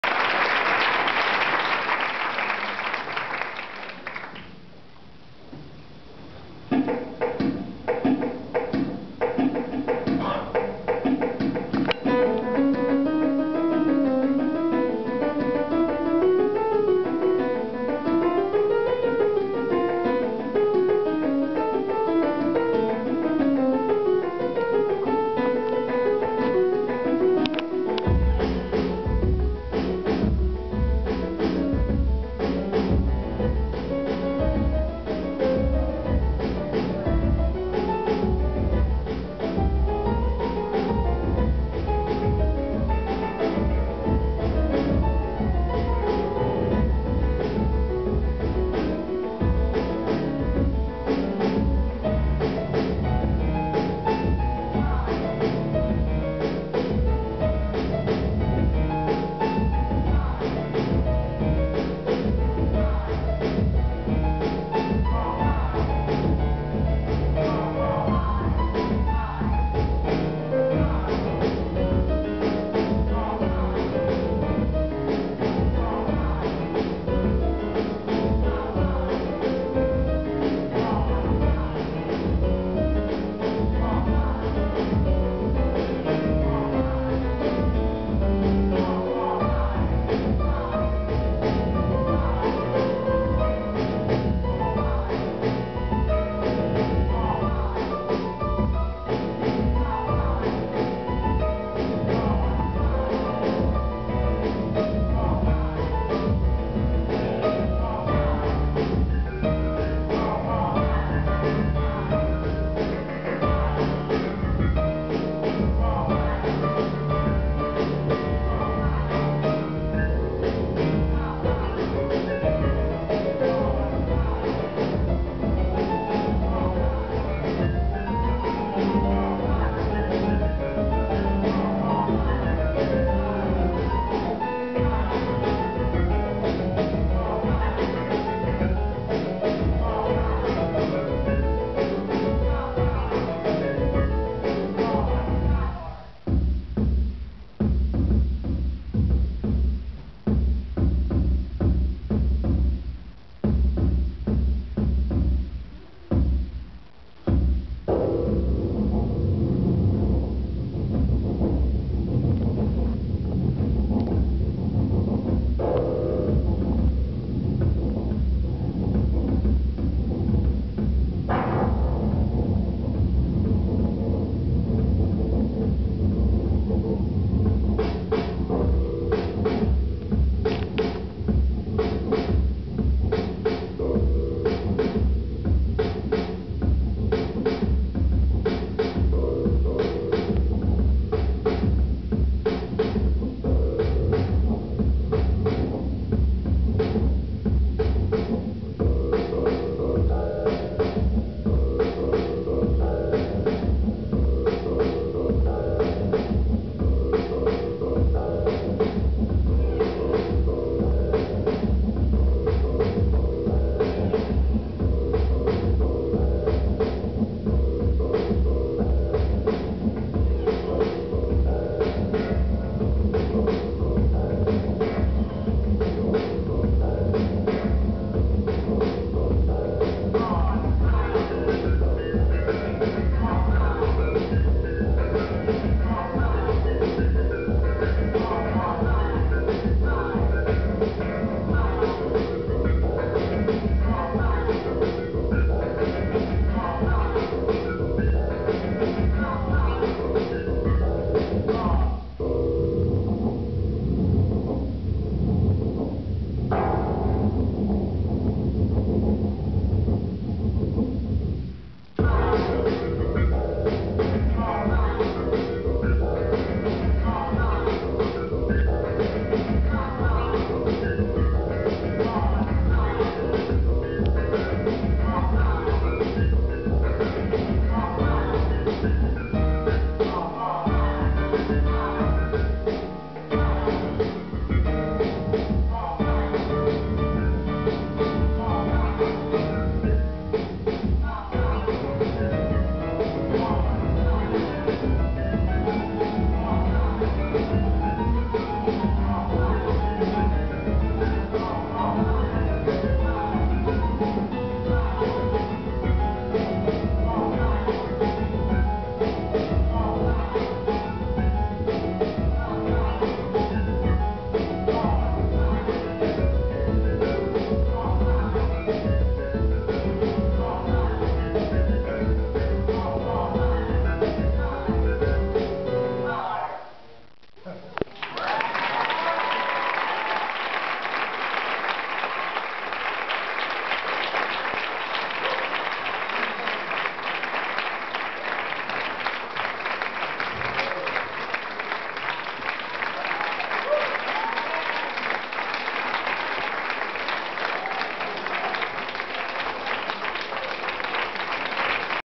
Jade Simmons and Roburt Reynolds Nominated Best Performance of 2009 Houston Press
Debut concert for Jade's "REvolutionary Rhythm". This duo will be recording new tracks in the summer of 2010 for her sophomoric Koch Records Release... for more updates, tracks, photos, and details! Video by Melissa Noble.